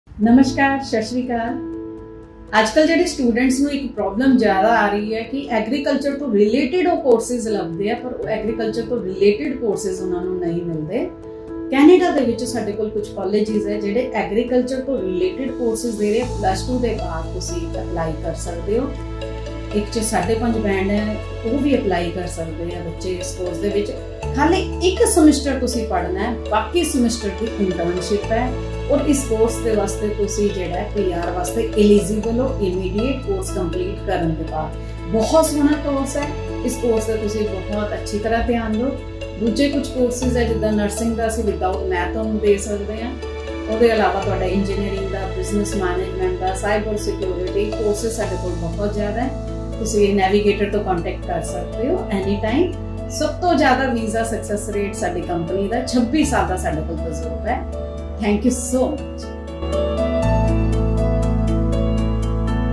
Canada Study Visa - Apply For Sept. 2023 Intake - Navigators Overseas - Veena Goel
Unlock your potential with Canada's world-class education system. Apply for a study visa and discover endless opportunities in Agriculture, Nursing, Cybersecurity, Engineering, and Business Management

👉 IELTS 6 not less than 5.5 Bands

📍 Sco 437-438, Second Floor, Sector 35-C, Chandigarh.